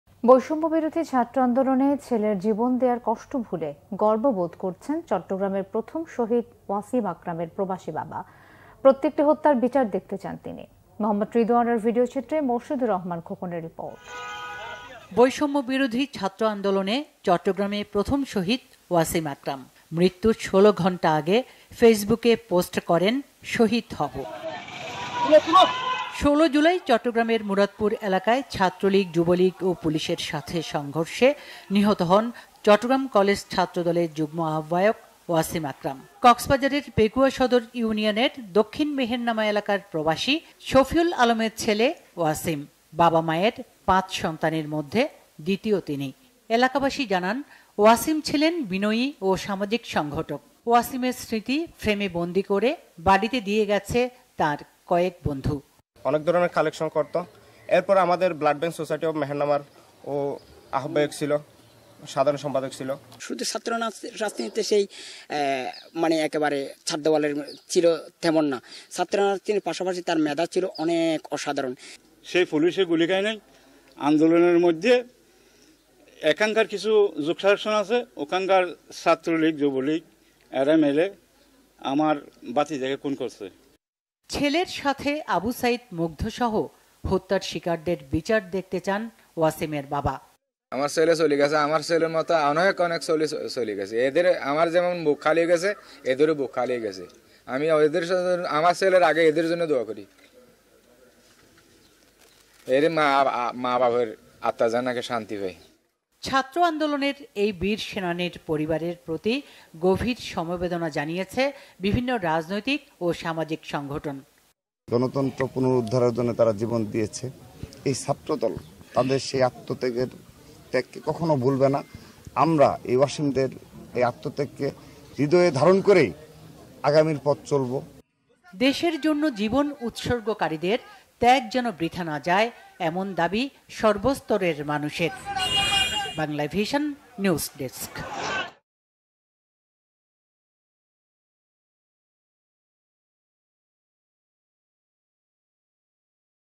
ছেলের জীবন দেওয়ার কষ্ট ভুলে গর্ববোধ করছেন ওয়াসিমের প্রবাসী বাবা
ছেলের জীবন দেওয়ার কষ্ট ভুলে গর্ববোধ করছেন ওয়াসিমের প্রবাসী বাবাBanglaVision is the most popular TV channel in Bangladesh that offers unbiased & authentic news and entertainment programs.

BanglaVision HQ:
Dhaka-1205, Bangladesh.